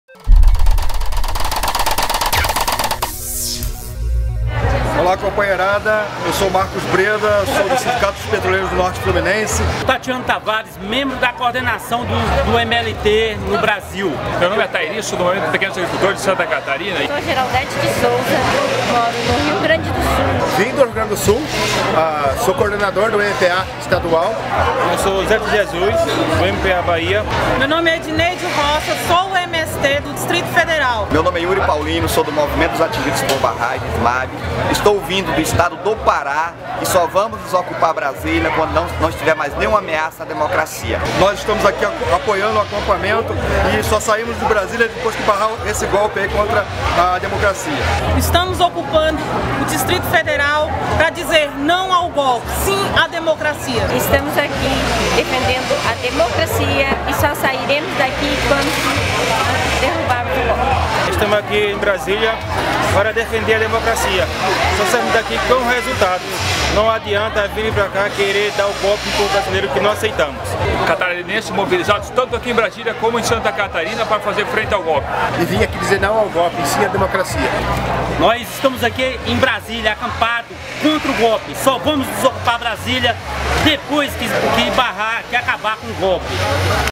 NãoVaiTerGolpe: Um acampamento com a cara e a coragem do Brasil
Brasileiros de todos os matizes, credos e regiões explicam porque são contra o golpe e cobram respeito à democracia e aos seus direitos.